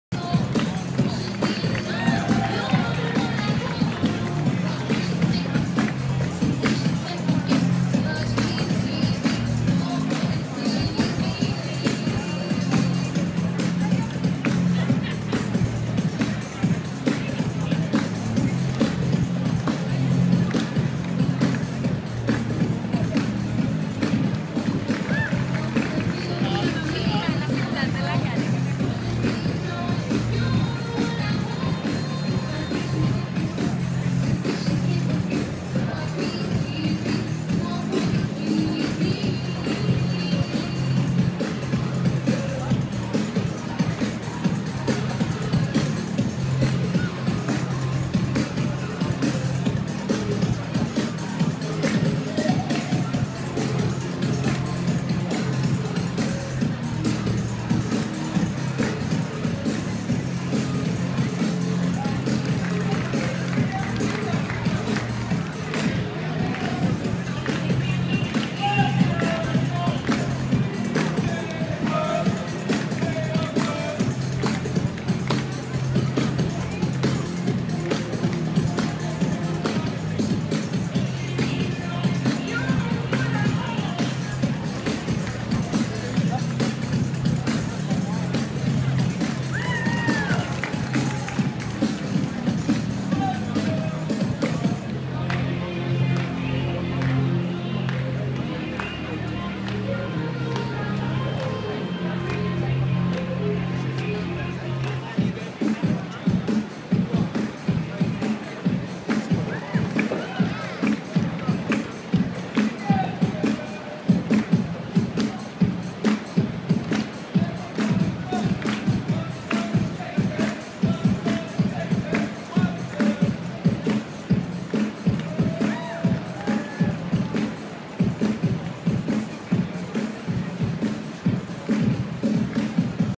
Times Square, Broadway NYC - 2013 - Feel the Energy!
BROADWAY& TIMES SQUARE, NEW YORK - Feel the energy - Like Disney's Main Street Party but unscripted! The Big Apple. Times Square. Traffic  gone. A pedestrian mall now, with cartoon creatures, entertainers, neon, Nasdaq, & pure Big Apple Energy.